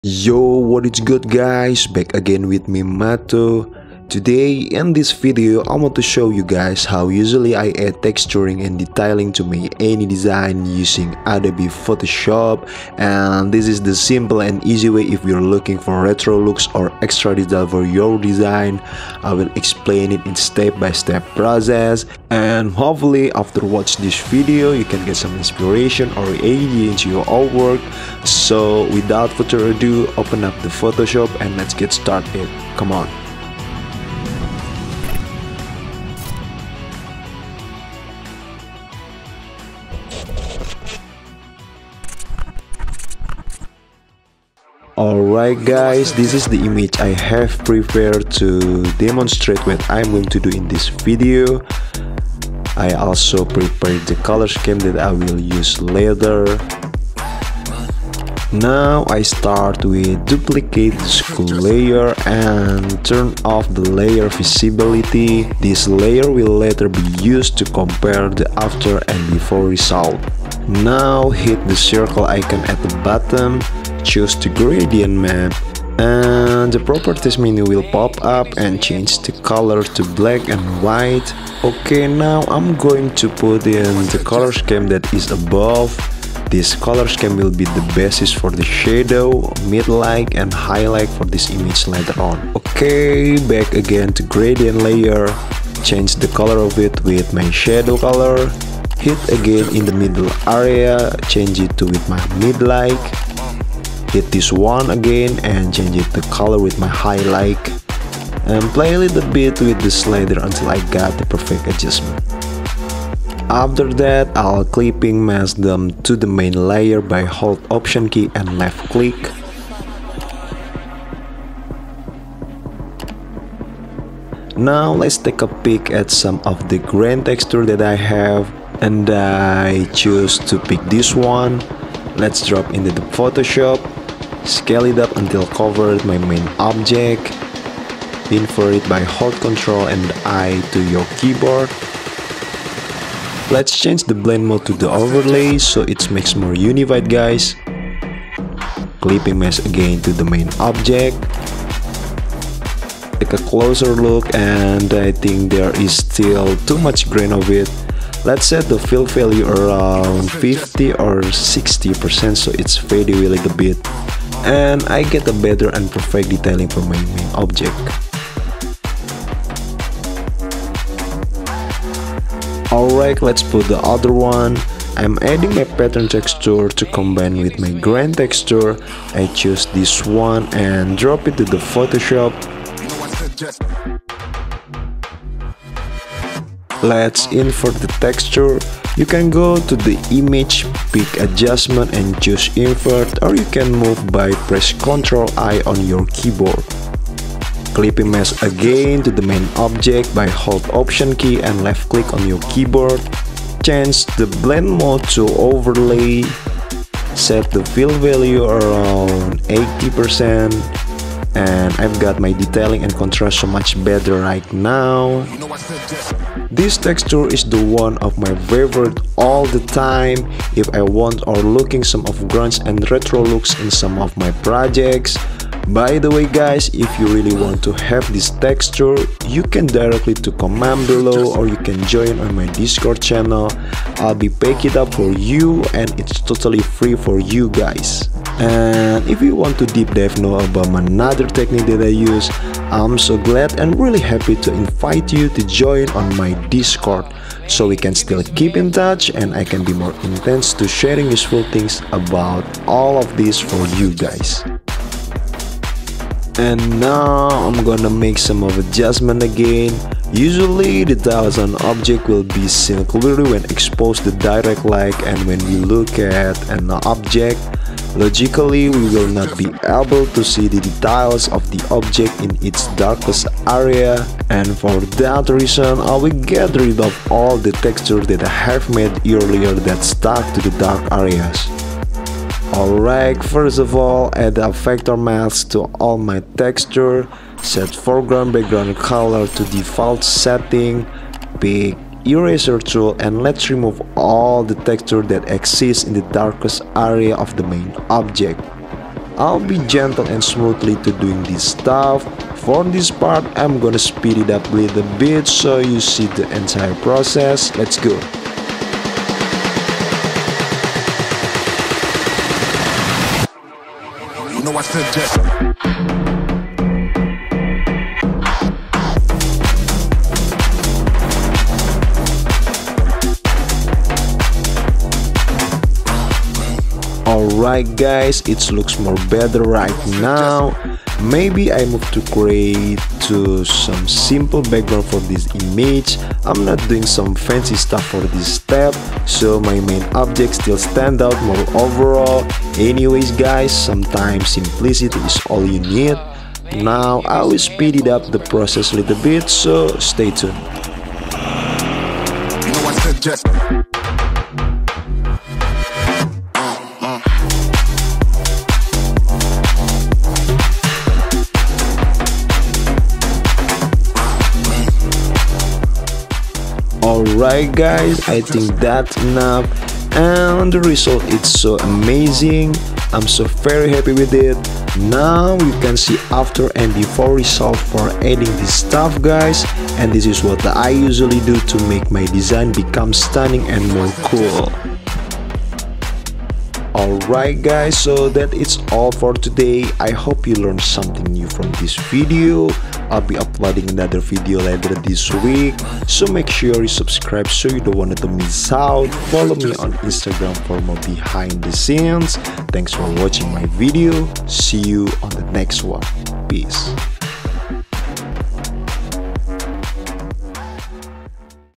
Make Your Design Look Retro By Add Texture For Any Images Using Photoshop (Part 1)
Hello guys, Watch me adding texture and detailing any images in Photoshop! This is my ultimate way to make my design more stuning and edgy. i hope you can learn something new from my videos, and hopefully it can be increase your skill to next level.
Regards, Matto.

its makes a difference. i really appreciate for your support guys.